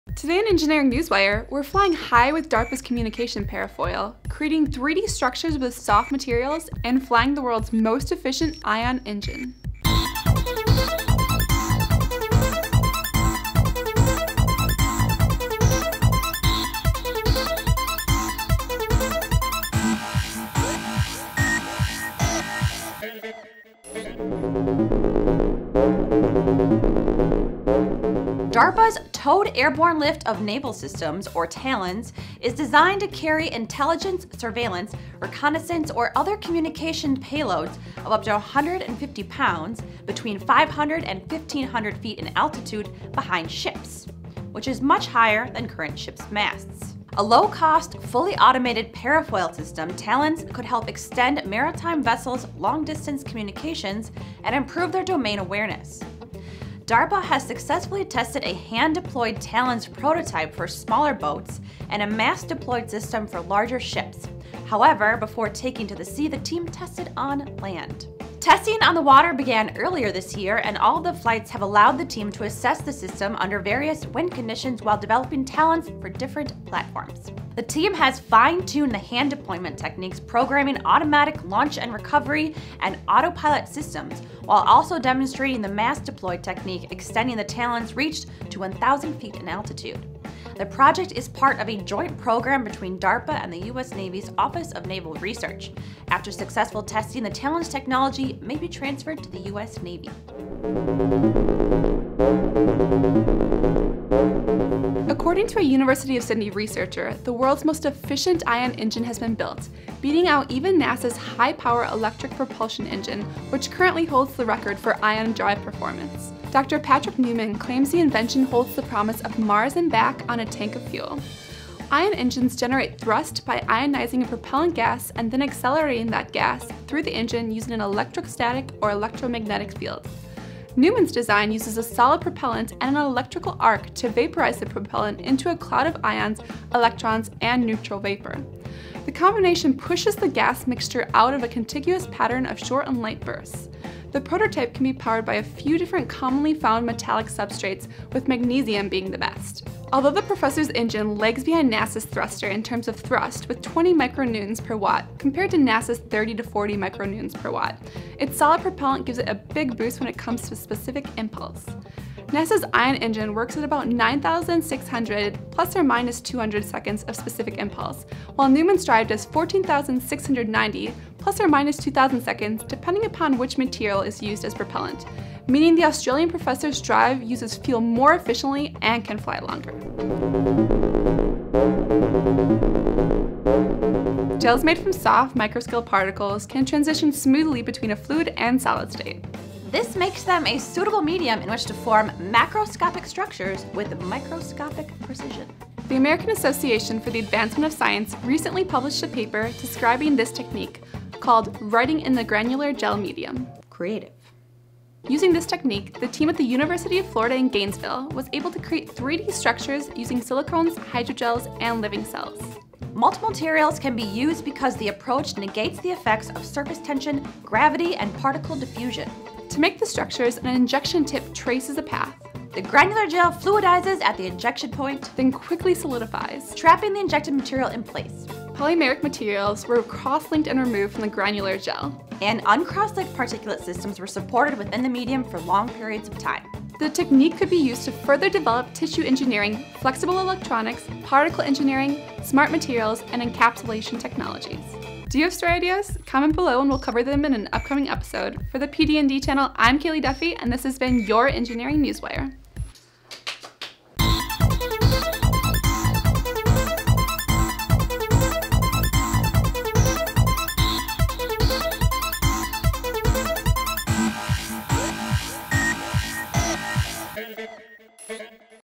Engineering Newswire 159: The World's Most Efficient Ion Engine
Today on Engineering Newswire, we’re flying high with DARPA’s communications parafoil, creating 3D structures with soft materials, and developing the world’s most efficient ion engine.

DARPA’s Communication Parafoil

DARPA’s Towed Airborne Lift of Naval Systems, or TALONS, is designed to carry intelligence, surveillance, reconnaissance, or other communications payloads of up to 150 pounds between 500 and 1,500 feet in altitude behind ships.

A low-cost, fully automated parafoil system, TALONS could help extend maritime vessel’s long-distance communications and improve their domain awareness. DARPA has successfully tested a hand deployed TALONS prototype for smaller boats, and a mast deployed system for larger ships. However, before taking to the sea, the team tested on land

World’s Most Efficient Ion Engine

According to a University of Sydney researcher, the world’s most efficient ion engine has been built – beating out even NASA’s High Power Electric Propulsion engine, which holds the record for ion drive performance. Dr. Patrick Neumann claims the invention holds the promise of “Mars and back on a tank of fuel.”

Ion engines generate thrust by ionizing a propellant gas and then accelerating that gas through the engine using an electrostatic or electromagnetic field. Neumann’s design uses a solid propellant and an electrical arc to vaporize the propellant into a cloud of ions, electrons, and neutral vapor.

Writing in Gel

Gels made from soft microscale particles can transition smoothly between a fluid and solid state. This makes them a suitable medium in which to form macroscopic structures with microscopic precision.

The American Association for the Advancement of Science recently published a paper describing this technique, called “Writing in the granular gel medium.”

Using this technique, the team at the University of Florida in Gainesville was able to create 3D structures using silicones, hydrogels, and living cells.

The technique could be used to further develop tissue engineering, flexible electronics, particle engineering, smart materials, and encapsulation technologies.